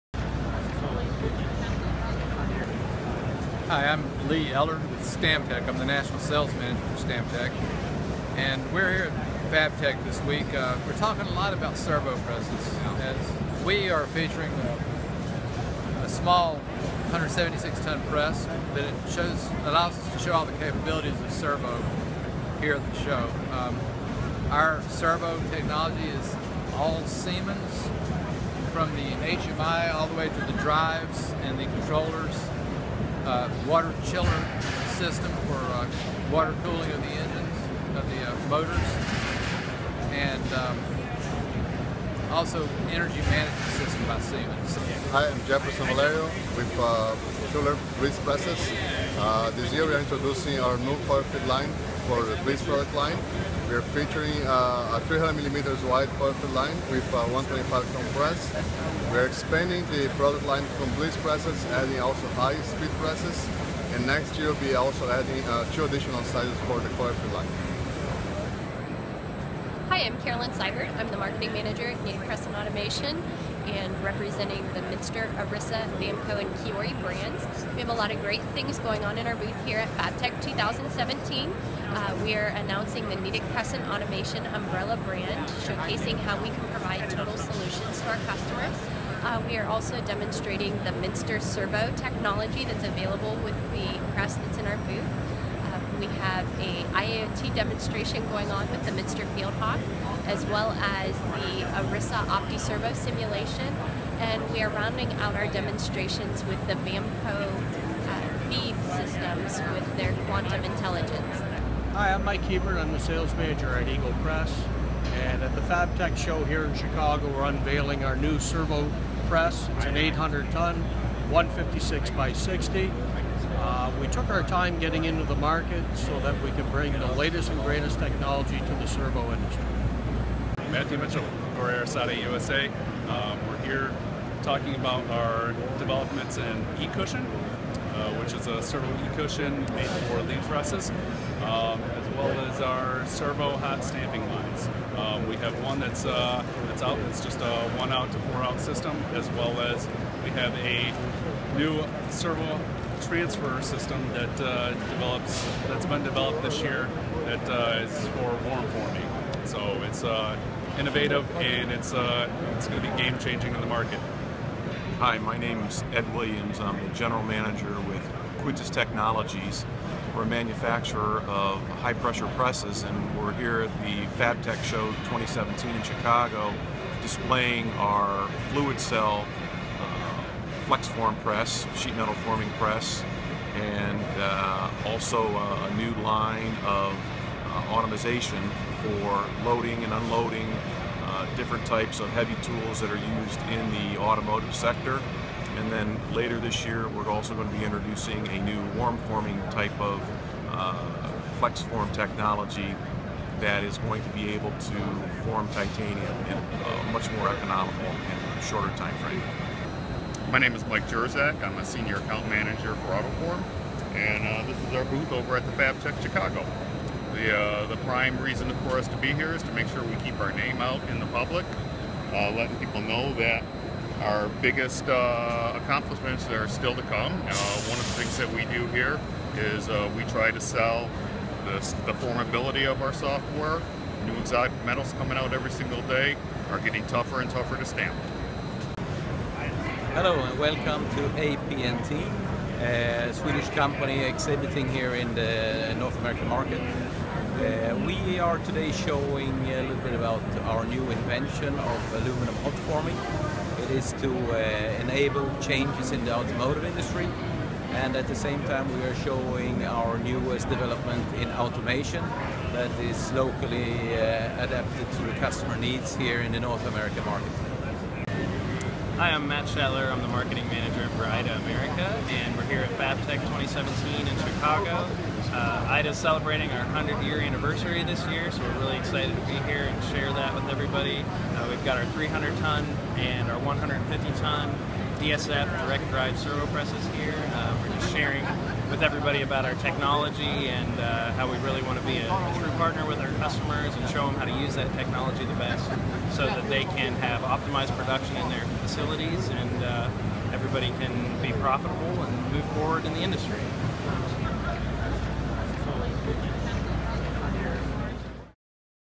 FABTECH 2017 Flashback
STAMPING Journal® Editor Kate Bachman stopped by some stamping OEM’s booths to speak with exhibitors at FABTECH17 and relay what they brought to the show floor.